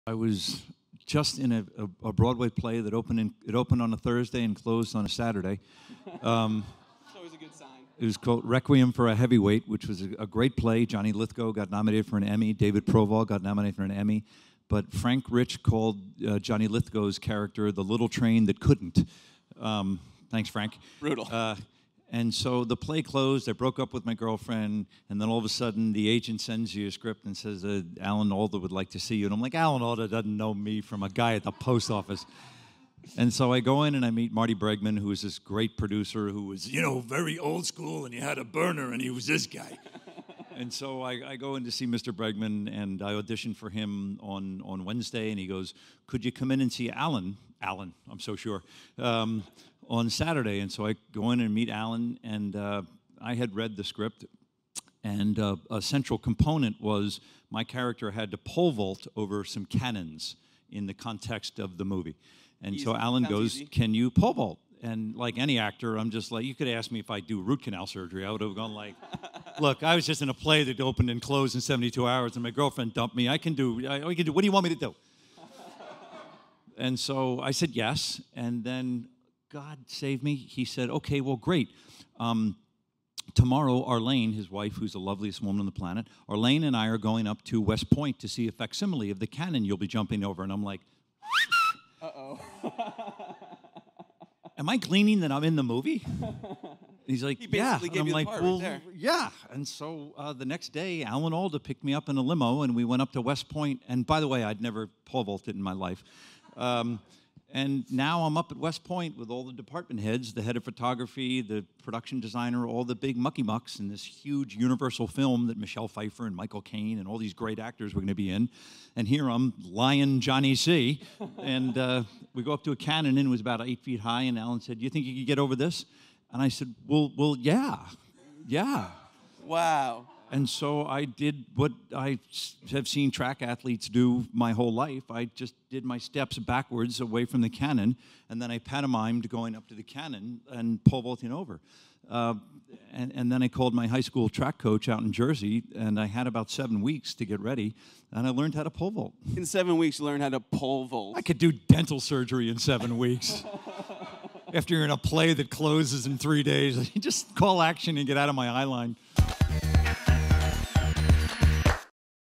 John C. McGinley Talks About Pole Vaulting Over A Cannon For Alan Alda | BUILD Series
John C. McGinley talks about pole vaulting over a cannon for Alan Alda. Interview at AOL HQ in NYC for BUILD Series.